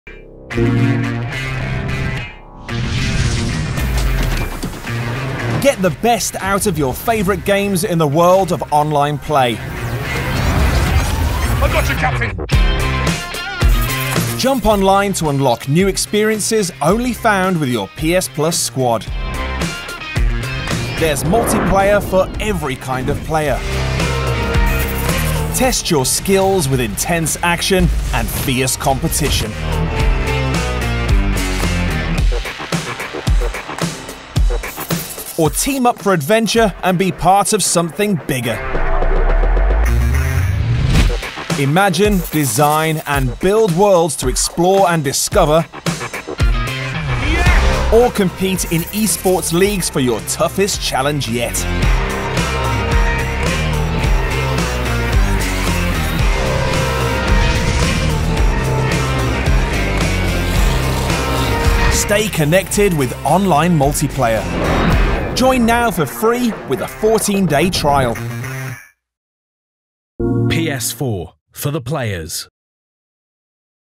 Discover Online Multiplayer | PlayStation® Plus
There is multiplayer for every kind of player, no matter your skill you can jump online and join the fun. Discover multiplayer today with PlayStation® Plus